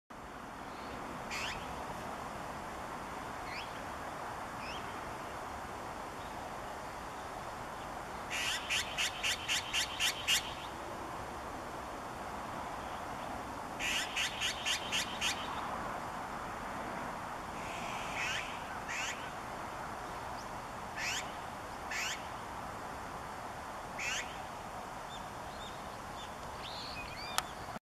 オナガ@鳴き声・グゥエグゥエ鳴いていました！
オナガが近くで例のグゥエグゥエを披露していました。